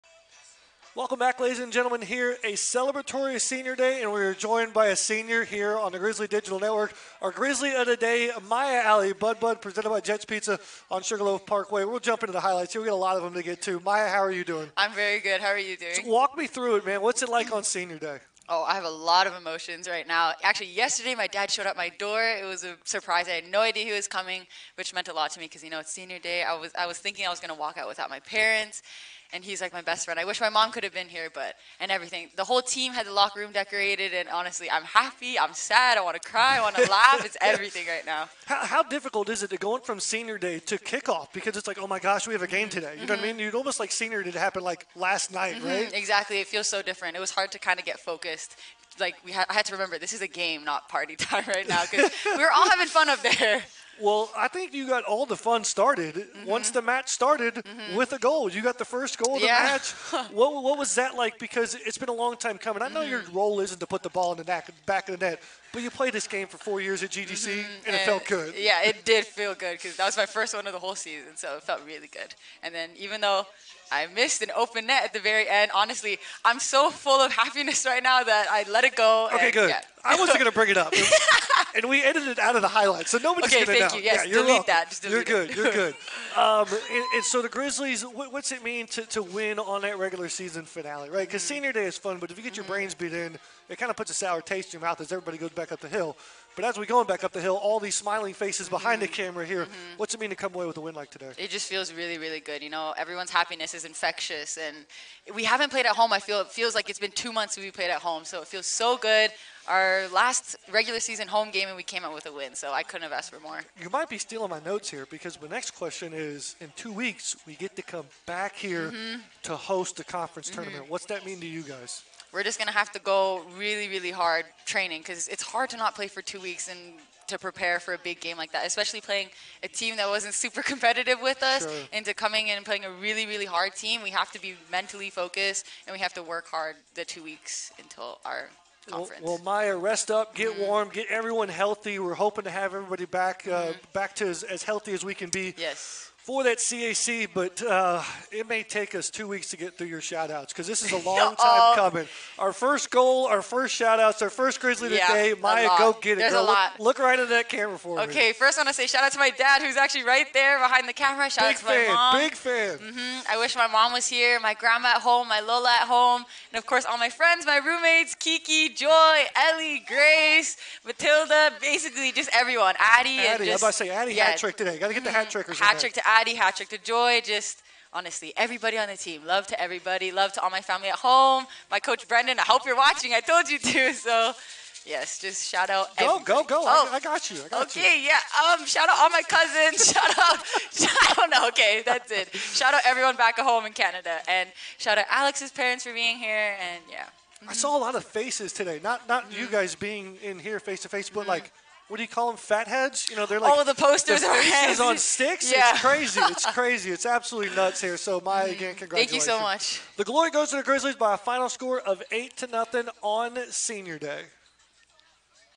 Grizzly of the Day: Maya Alibudbud vs Florida College
Presented by Jet's Pizza on Sugarloaf Parkway, post game comments and highlights from the Grizzlies victory over the Falcons